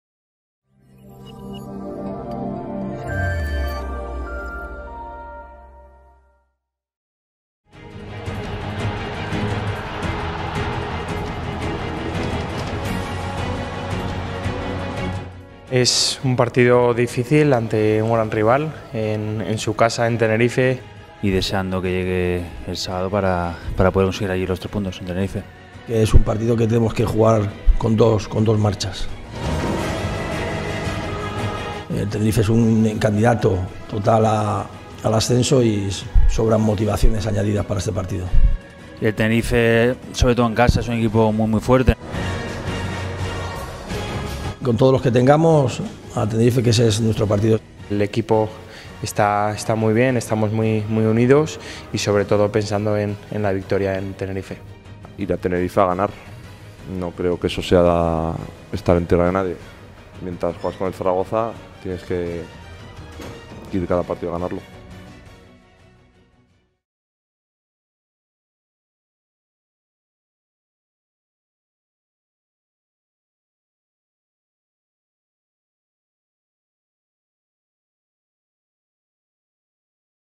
Previa: CD Tenerife - Real Zaragoza | 2/4/2021
Partido correspondiente a la jornada 34 de LaLiga SmartBank.